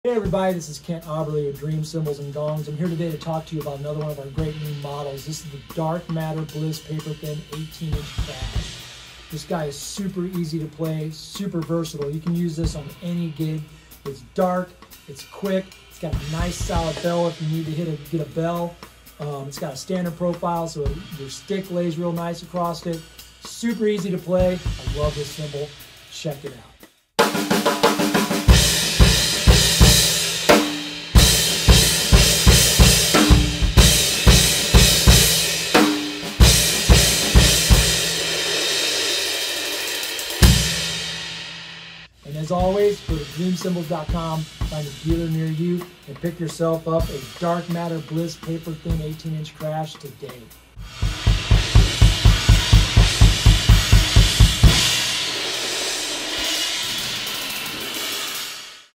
Dream Cymbals 18" Dark Matter Bliss Paper Thin with Kent Aberle
Dream Artist and Product Specialist Kent Aberle breaks down the 18" Dark Matter Bliss Paper Thin